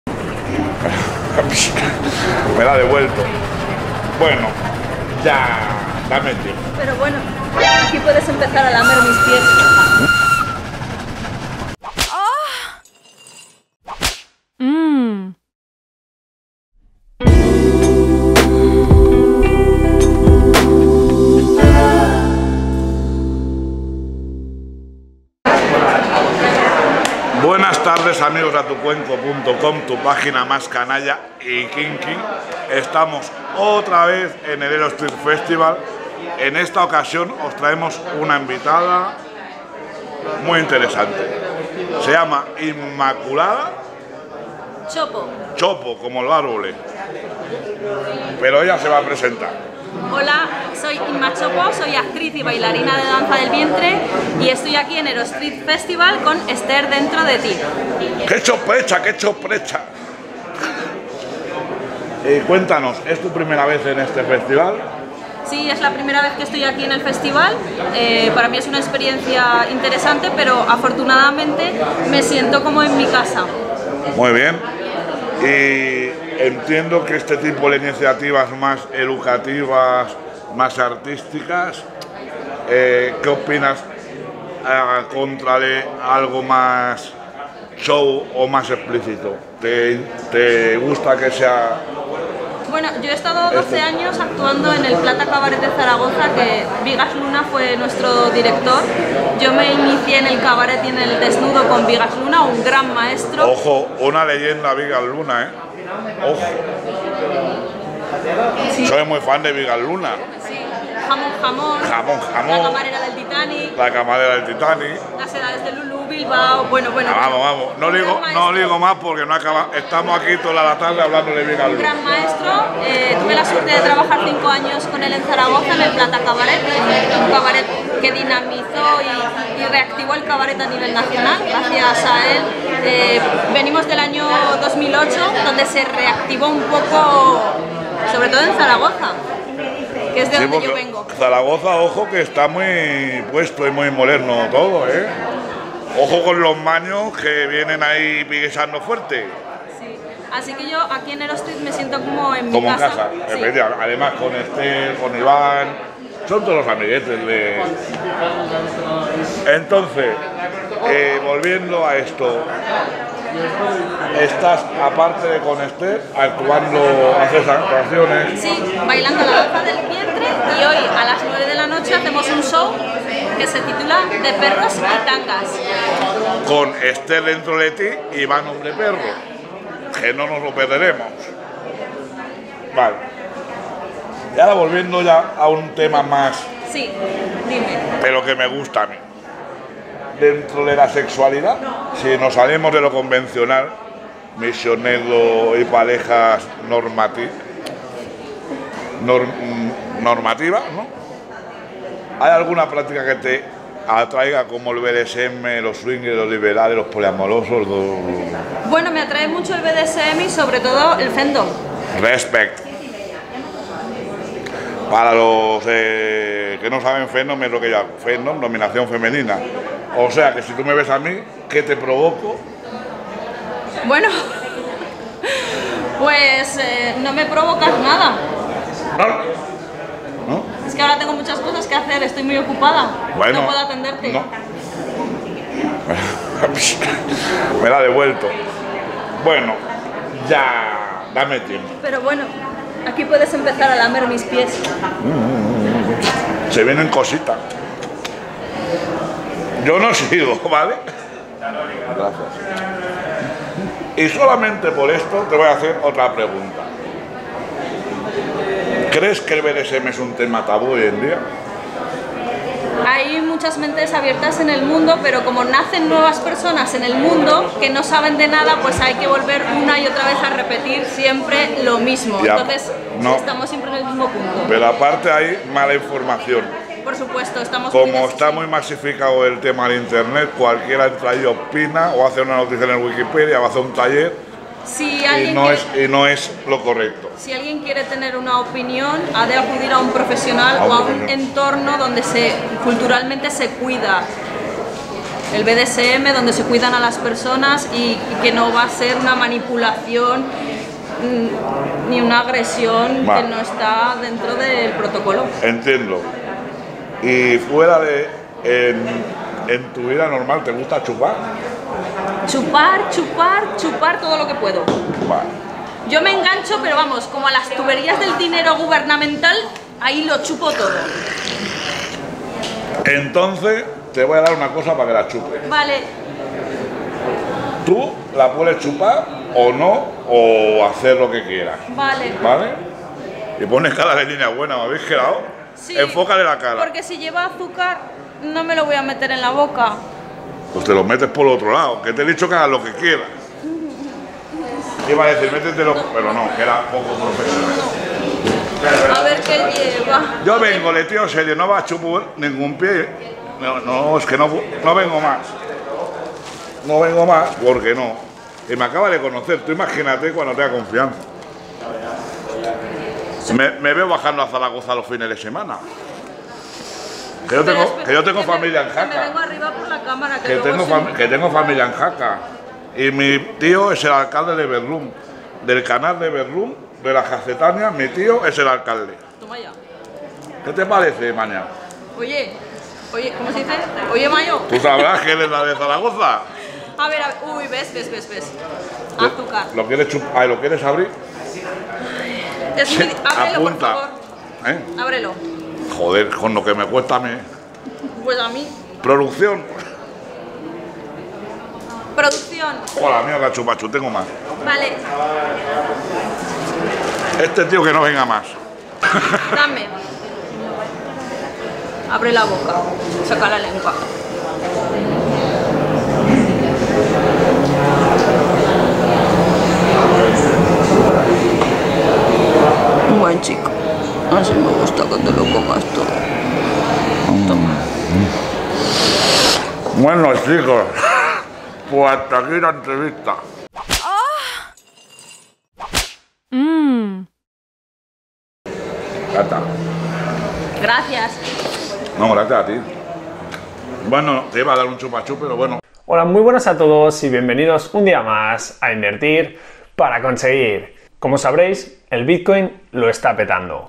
Inma Chopo, artista e inversora en criptomoneda | Erostreet Festival
Inma Chopo nos cautivó con su simpatía y humor, además de por su obra "De Perros y Tangas" que trajo el estand "Esther dentro de ti". Aprendimos con ella quien es Bigas Luna y cual había sido su recorrido artístico. Una "maña" que no debéis dejar de conocer y que esperamos volver a encontrarnos pronto. Además, para sorpresa de todos (incluido el cámara) una entendida de las criptomonedas.